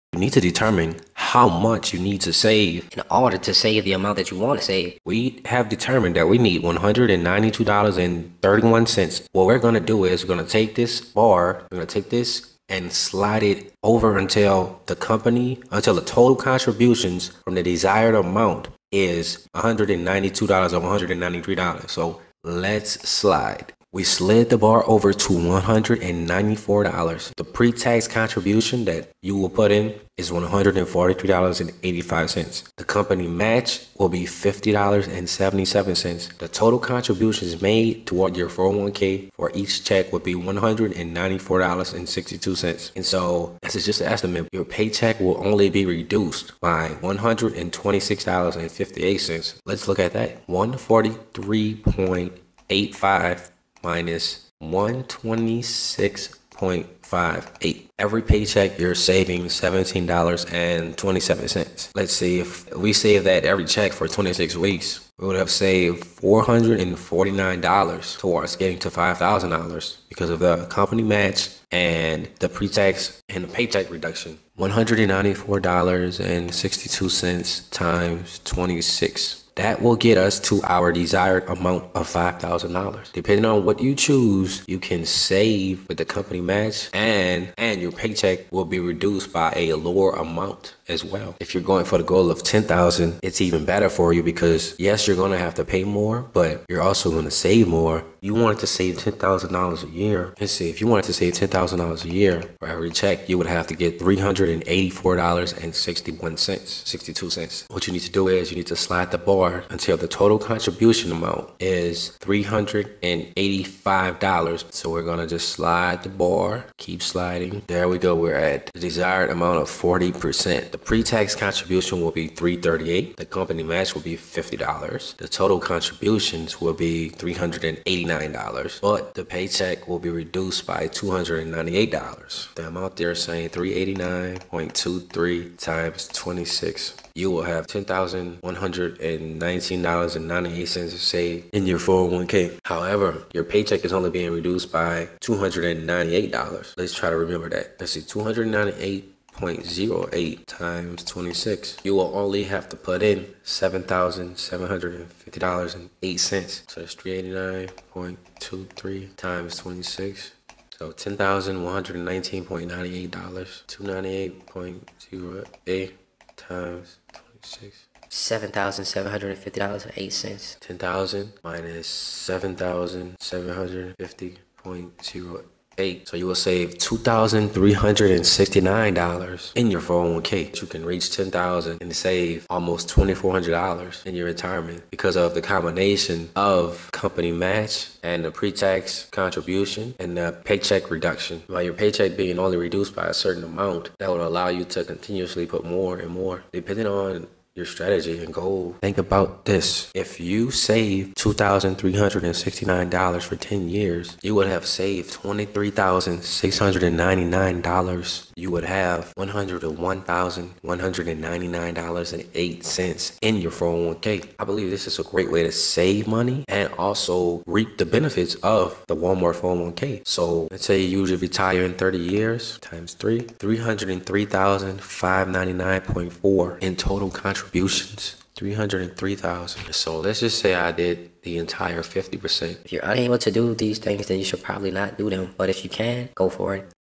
Walmart 401k with Merrill Lynch, How to Save $10,000 A Year for Retirement
Walmart 401k, How to Save $10,000 A Year for Retirement

Walmart 401k Plan with Merrill Lynch allows its employees to save money for retirement each pay period. Walmart makes a matching contribution of up to 6% of 50 percent of your eligible pay up to the contribution limits of $19,000. Whatever your 6% number is Walmart will match that number 100%.  If Your 6% number is $50, then Walmart will make a matching contribution of $50. Walmart also has a Roth 401k for after tax contributions.

Pre-tax contributions allow you to contribute and have more take home pay. With the employer match & Pre tax contributions, you can maximize the amount you save in your 401k. The amount you add to your 401k varies based upon your salary, but the more you add the more you'll save and have in your 401k.

How to Check Your 401k Balance & Change Contribution Rate:

1. Visit Benefits.ml.com
2. Log in to Your Account
    (Walmart 401k Plan)
4. View Your Account Summary
5. Click Current Elections
6. Choose Contribution Rates
7. View Your Pre-tax Contribution Rate
8. Click change Contribution Rates
9.Enter/Update your Annual Salary and Payroll Frequency to model
    your per pay period contributions, and to see how pre-tax
    contributions could affect your take home pay.
10. Under pre-tax contribution select the desired amount to add to
   your 401k plan.
11. You can adjust the pretax contribution amount up to 50% eligible pay, but the company match only goes up to the 6%.
12. View Your total contribution amount per check.
13. View how much your paycheck will be reduced by after 401k is deducted.
14. These numbers are all estimates, and can be adjusted each paycheck, if changed in a timely manner.

To save $5,000 a year for retirement you need to save $193 per paycheck biweekly. To save $10,000 you need to save $385 per paycheck biweekly. Under Pre-Tax 401(k) Contribution Slide the bar to the desired amount until the total contributions under the desired section reaches $193 or $385. You can put more to ensure that you reach the total desired amounts of $5,000 & $10,000. You can also contribute to a Roth 401k, but the numbers to reach 5k or 10k might be different. Creating a balance between the account and making the right investment choices can help towards building a better future for retirement.

You may qualify for a savers credit and receive a tax credit for saving in your 401k. The tax credit is up to $2,000.